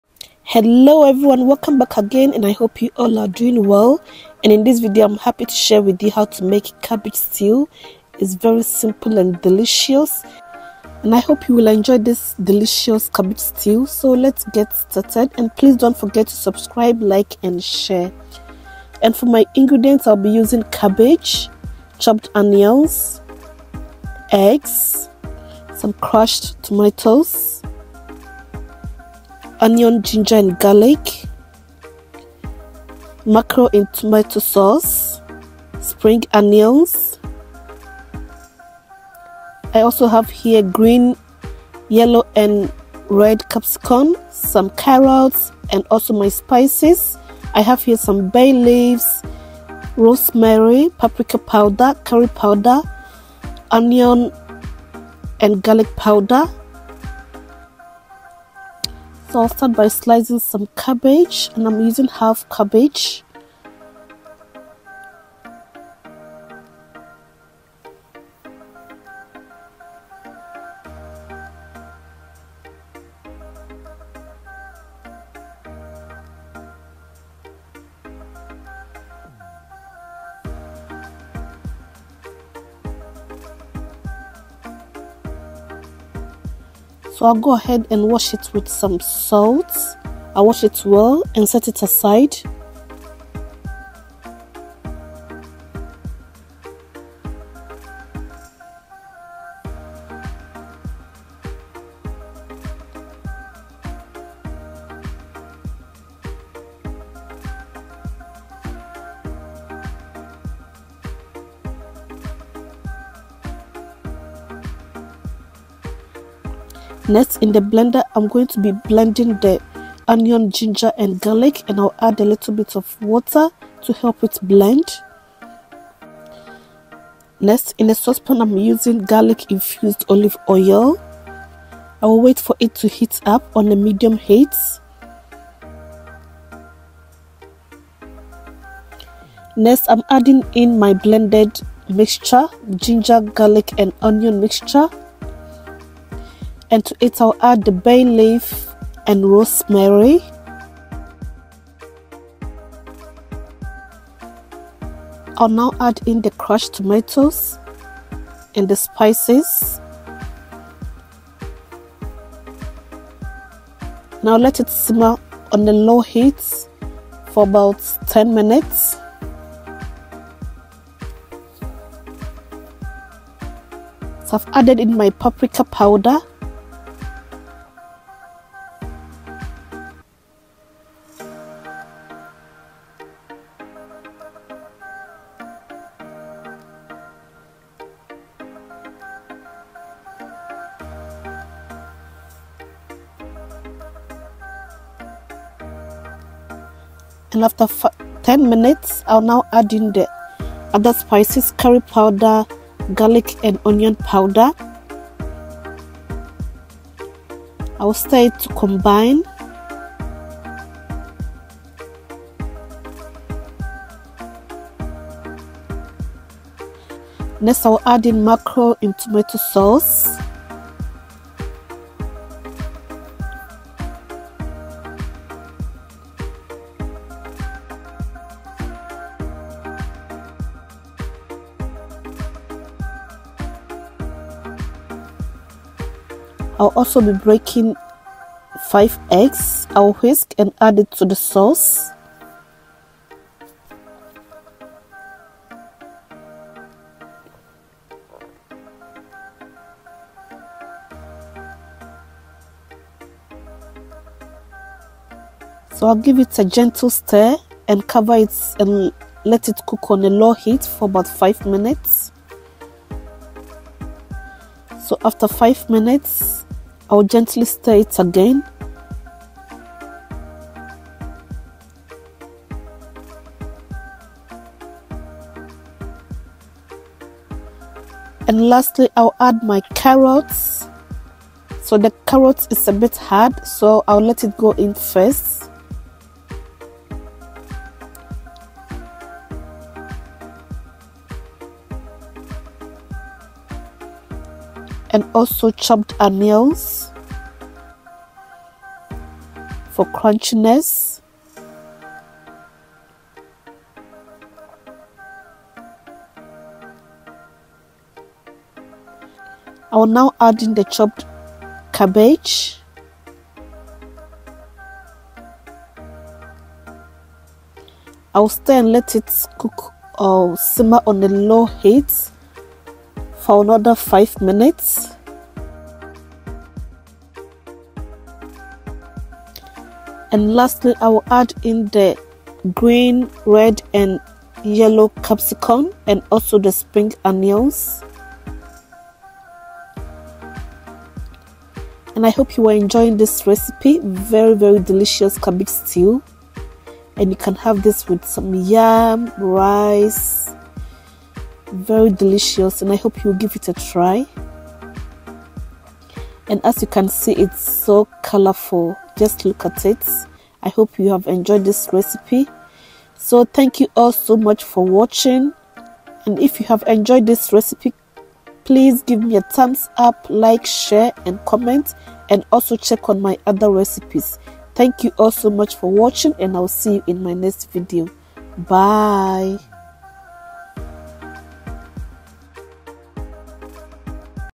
How To Make Tasty Ghana Cabbage Stew
Ingredients

1/2 Cabbage
Crushed Tomatoes
Chopped Onions
Spring onions
Blended ginger, garlic and Onion
Red, yellow and green capsicum
Mackerel in tomato sauce
1 tablespoons of curry powder
1 tablespoons of Chilli powder
1 tablespoon of paprika powder
1 tablespoons of vegetable seasoning
1 tablespoon of homemade spice blend
Garlic and ginger powder

Hope you have enjoyed this recipe.